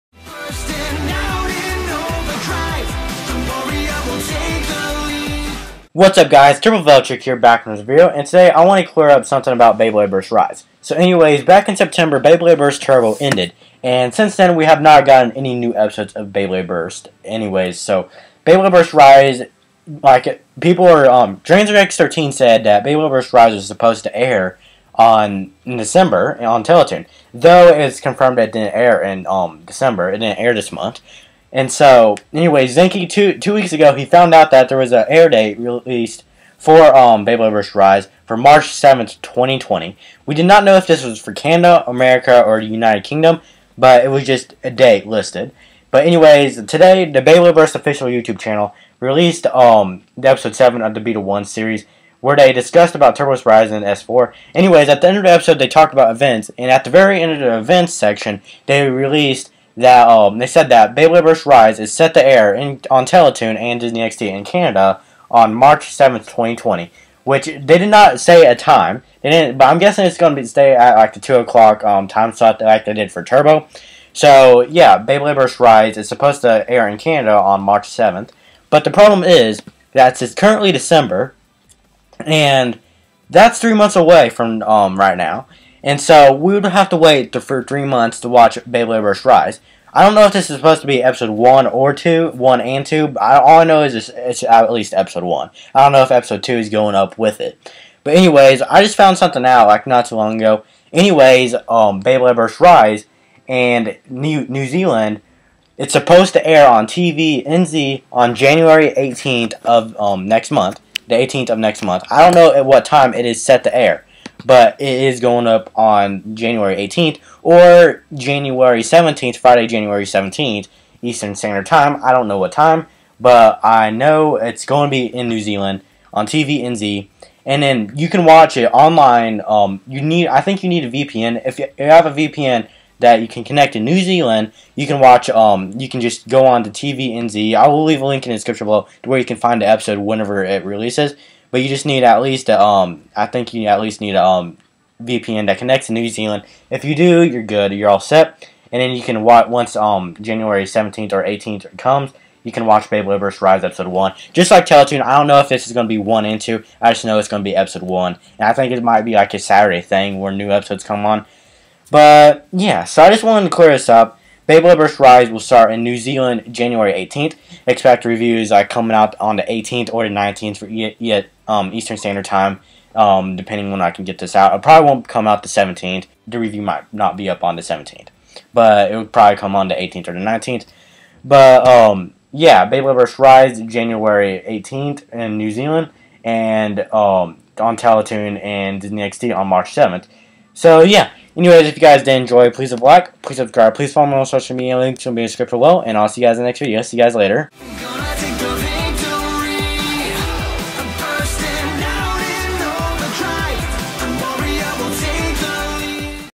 Beyblade Burst Rise: Official New Zealand and Canadian Release Dates
This Video is An Update Discussing The New Zealand and Canadian Release Dates for Beyblade Burst Rise.
Well It is Finally Coming!